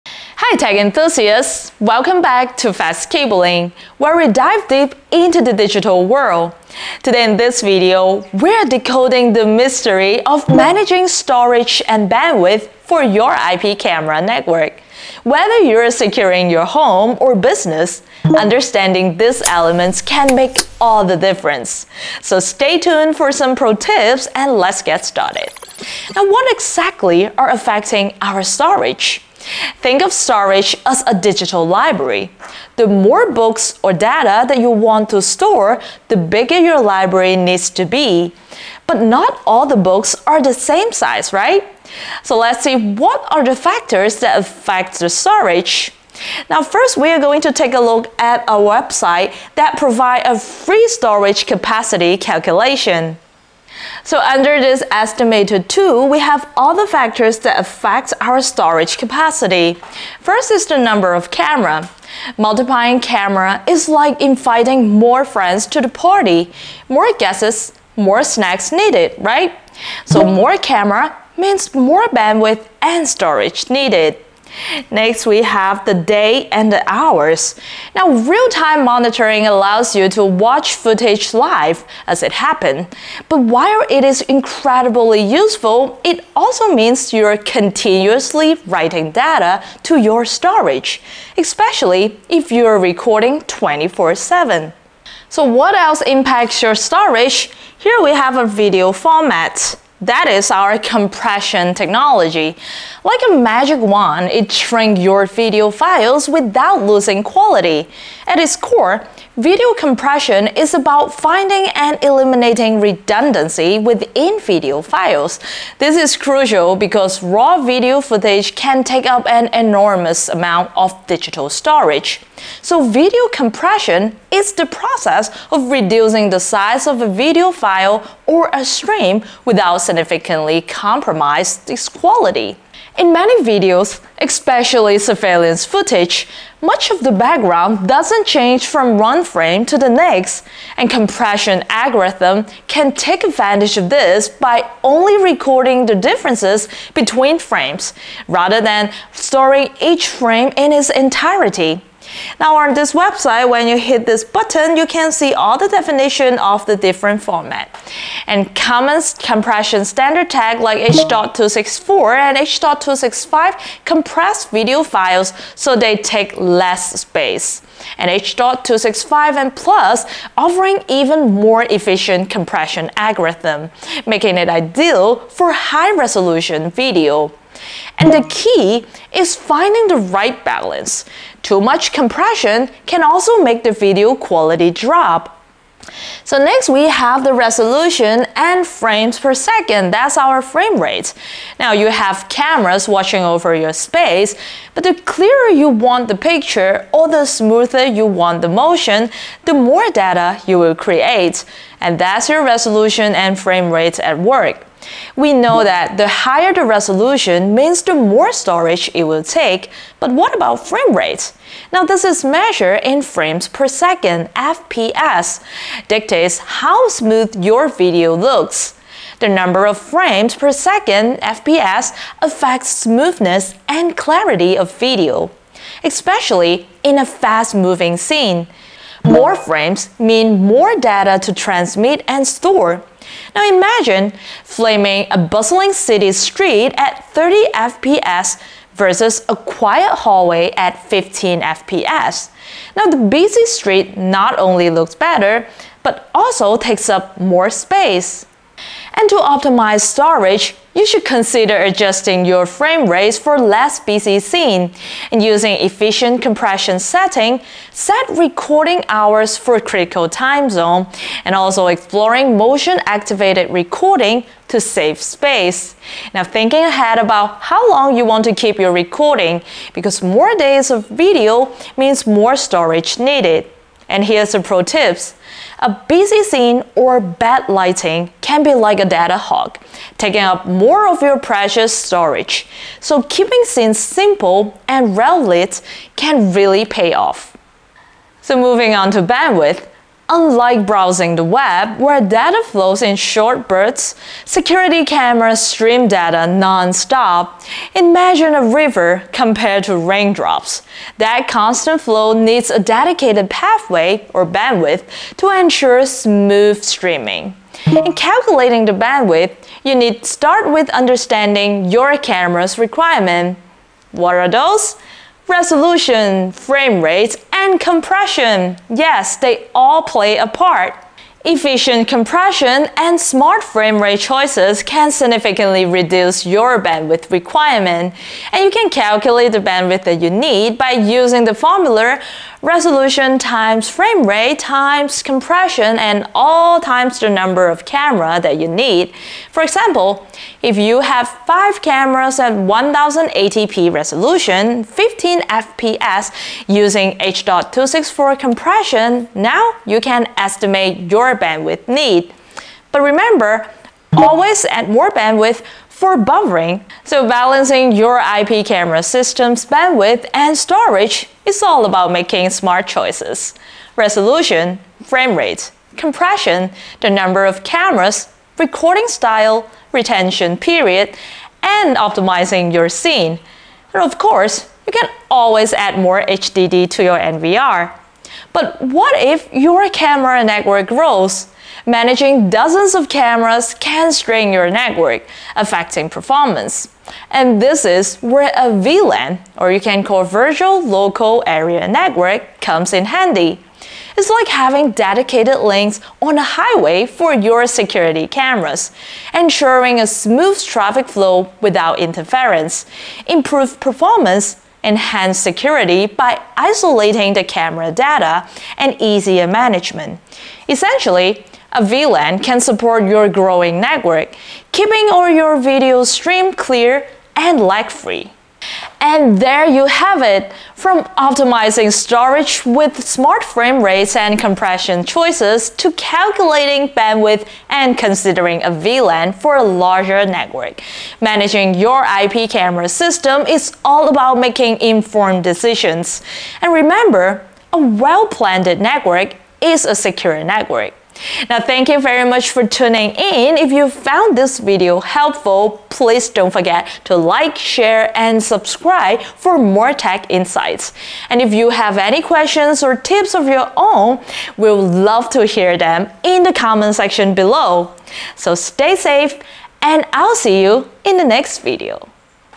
Managing Bandwidth & Storage for IP Camera Networks
Welcome to our comprehensive guide on managing bandwidth and storage for your IP camera network. In this video, we delve deep into the essentials of optimizing your IP camera setup for efficient storage without compromising on video quality. This guide will help you make informed decisions to ensure smooth operation and effective monitoring of your IP camera network.

0:00:00 The Media to Storage the Video of IP Camera
Factors to Consider: Bandwidth & Storage
Hard Drive Disk Choosing Guide
How to Calculate the Required Storage Space?